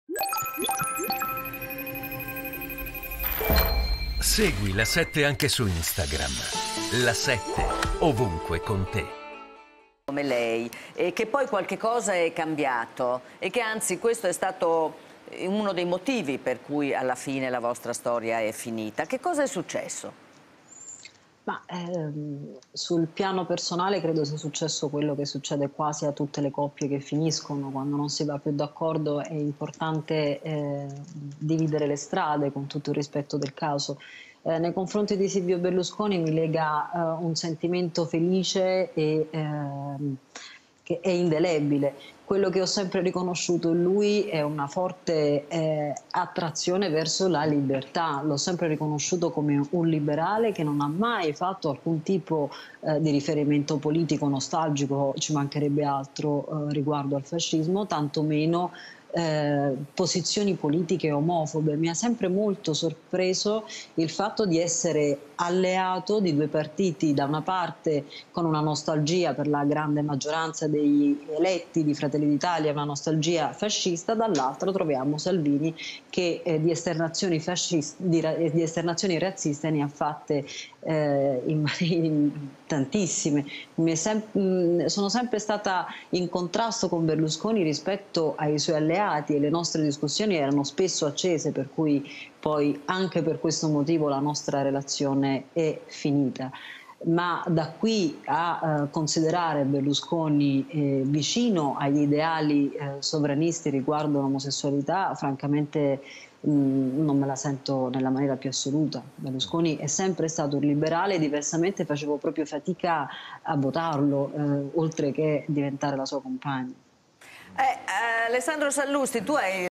Pascale su Berlusconi: "In contrasto con lui rispetto ai suoi alleati. Anche per questo motivo ...
Francesca Pascale sulla storia finita con Silvio Berlusconi: "Mi lega un sentimento felice e indelebile, lui è un liberale, sono sempre stato in contrasto rispetto ai suoi alleati. E le nostre discussioni erano spesso accese, anche per questo motivo la nostra storia è finita"